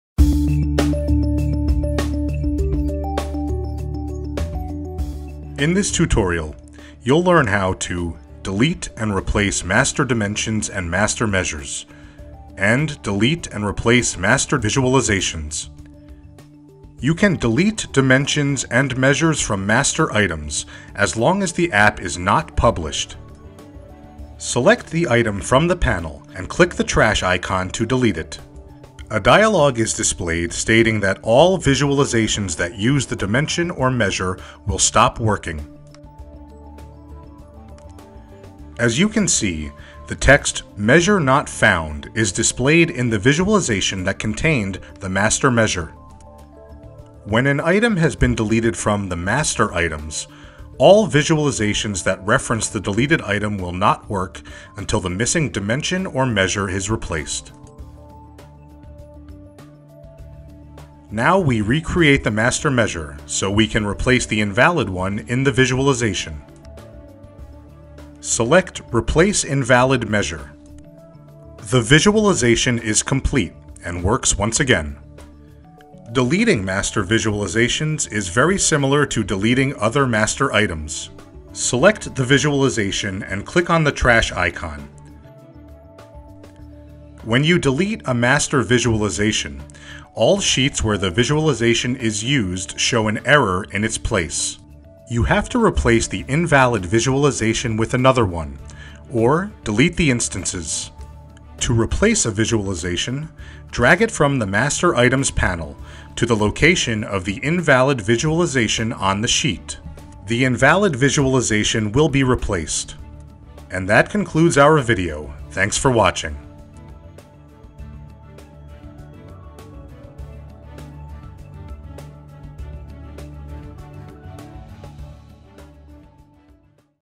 Deleting and replacing master items - Qlik Sense
In this video you’ll learn how to delete and replace master dimensions, measures and visualizations in Qlik Sense.